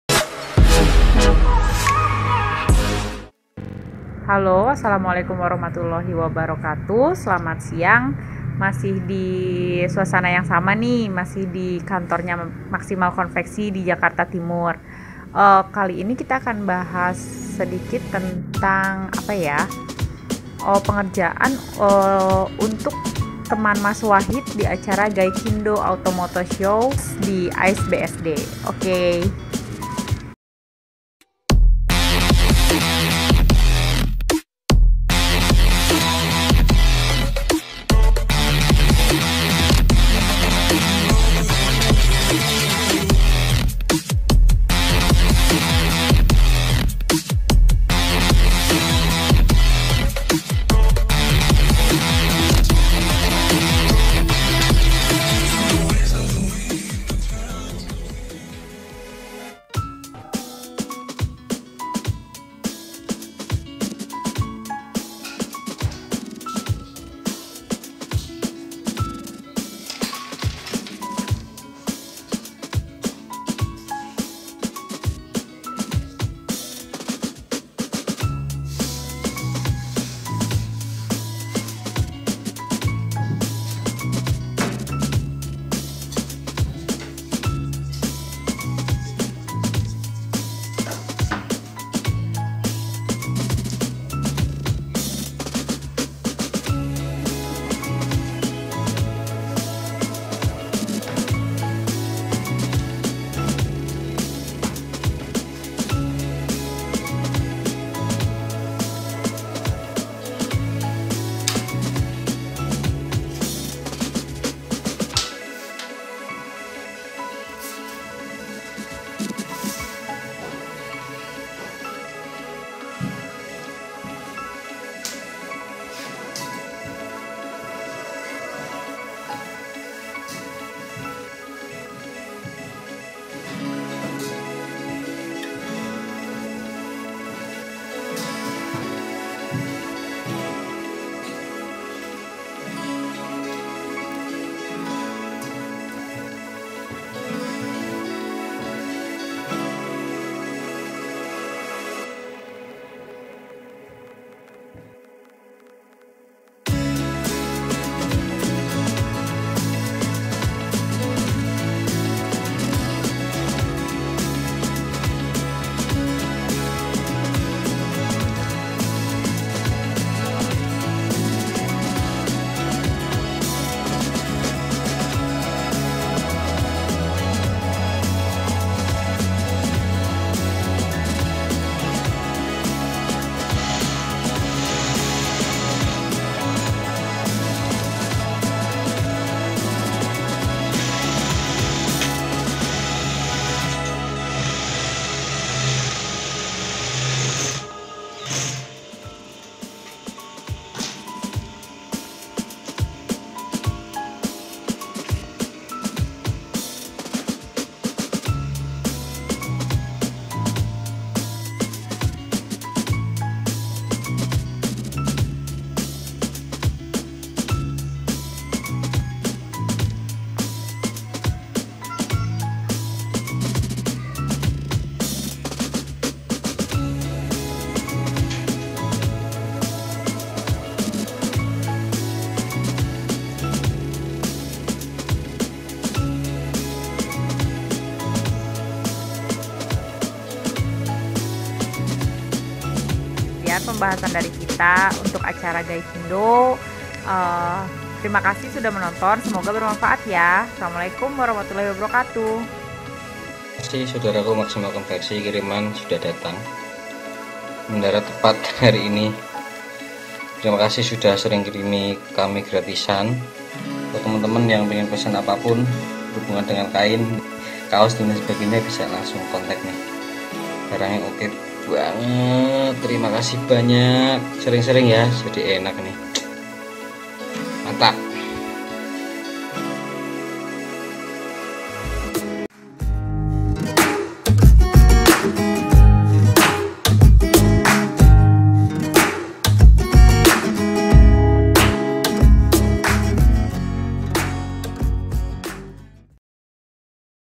Kaos Teman Mas Wahid
Review singkat mengenai pembuatan Kaos Teman Mas Wahid

info pemesanan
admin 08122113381